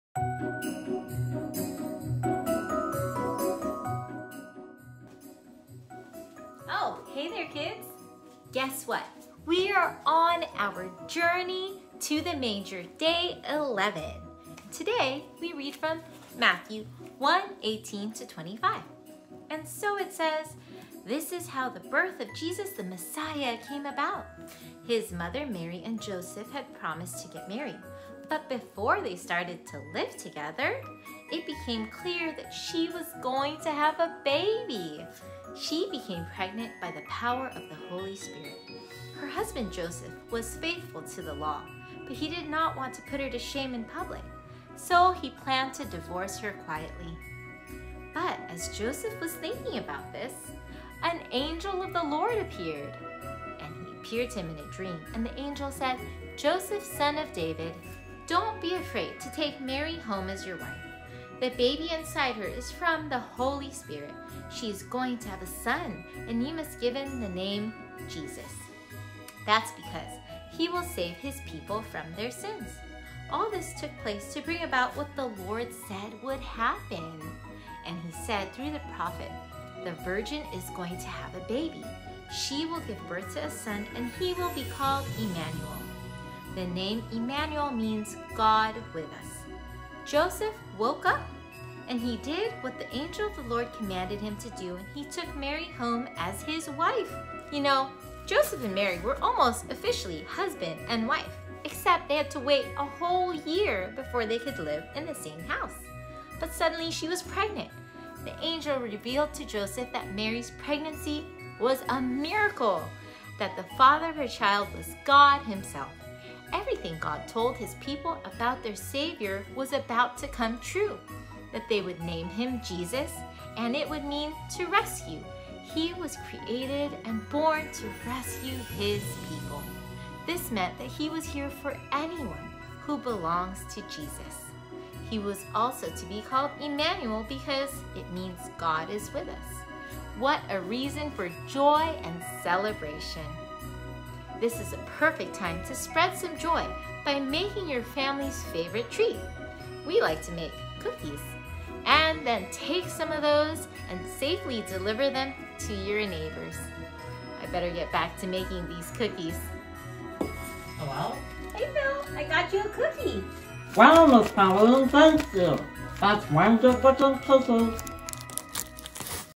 Day 11: Journey to the Manger
Day 11 of our 3CK Advent reading and challenge as we ⭐️ Journey to the Manger ⭐️ Follow along on our map at home, or you can find the map online to printout at 3Crosses.org/kids. 🌲
Pauleen shares today's reading from Matthew 1:18-25 along with today's challenge🌟Day 11: Advent Challenge - Make your favorite family treat and safely deliver them to neighbors!

Tag us using 3CKadvent on all the challenges you complete and don’t forget at the end of the month to send us a picture of your calendar to and you will be entered into a raffle to win a prize!
We can’t wait to go on this journey to the manger with you!